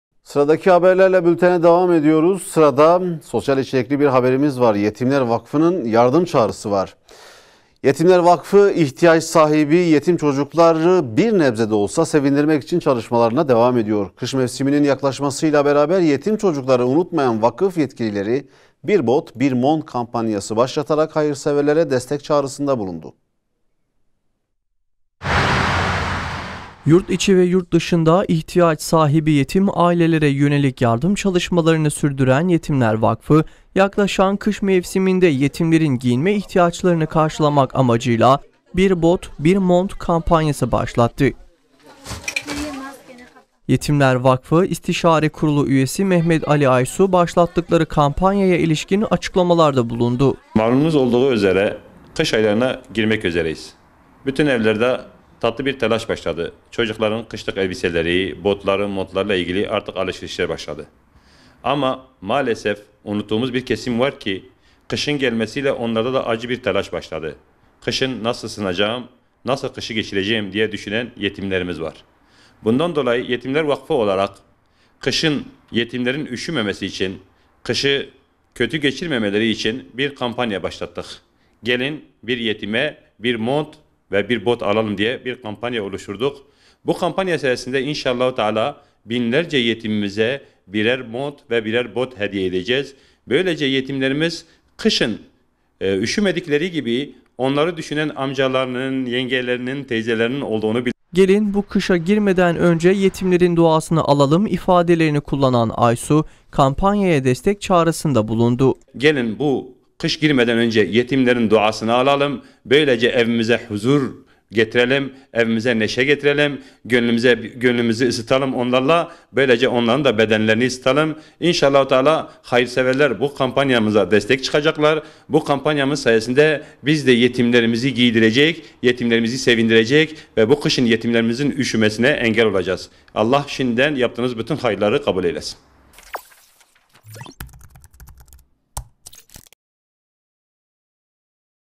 Yetimler Vakfı’ndan "Bir Bot, Bir Mont" Kampanyası
Yetimler Vakfı, ihtiyaç sahibi yetim çocukları bir nebze de olsa sevindirmek için çalışmalarına devam ediyor. Kış mevsiminin yaklaşmasıyla beraber yetim çocukları unutmayan Yetimler Vakfı, "Bir Bot, Bir Mont" kampanyası başlatarak hayırseverlere destek çağrısında bulundu.

   OYNATMA LİSTELERİMİZ